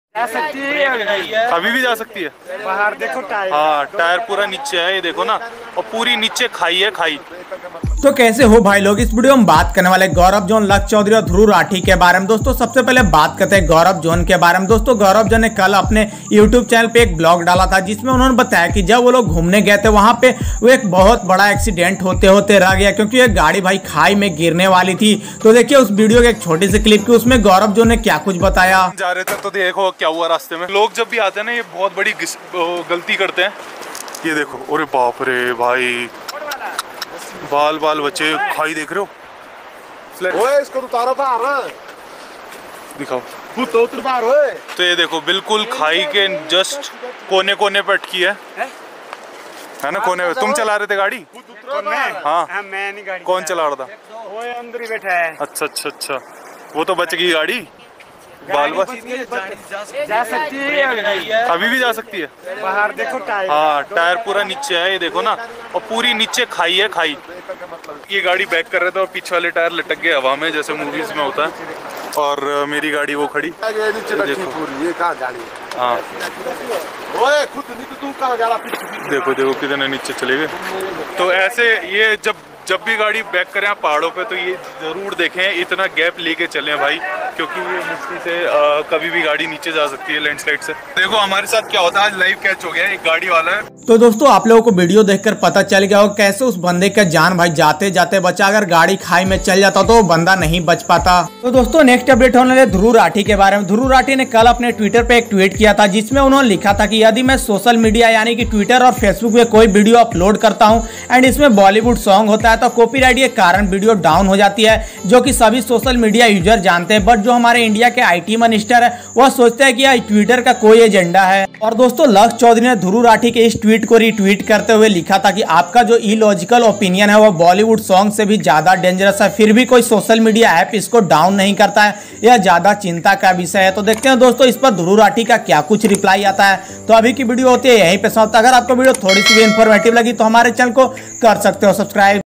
Gauravzone big accident | Lakshay chaudhary diss dhruv rathee
In this video we are going to talk about @GAURAVZONE  @lakshaychaudhary  and @dhruvrathee

Gauravzone in his recent video told that when he went on traking a big accident in happens on the road side that you can see in this video

Lakshay Chaudhary tease Dhruv rathee for his recent Tweet on India's IT minister Mr Ravishankar Prashad